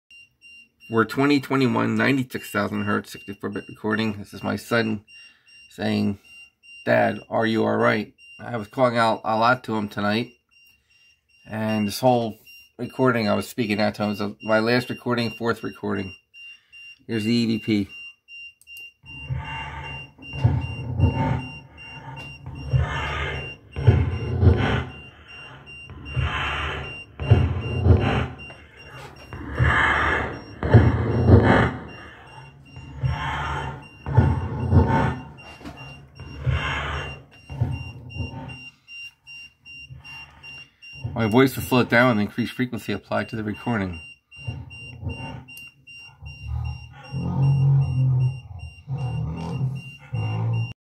12. My son Jeremy’s EVP asks “Dad are you alright?”
My son Jeremy’s EVP asks “Dad are you alright?”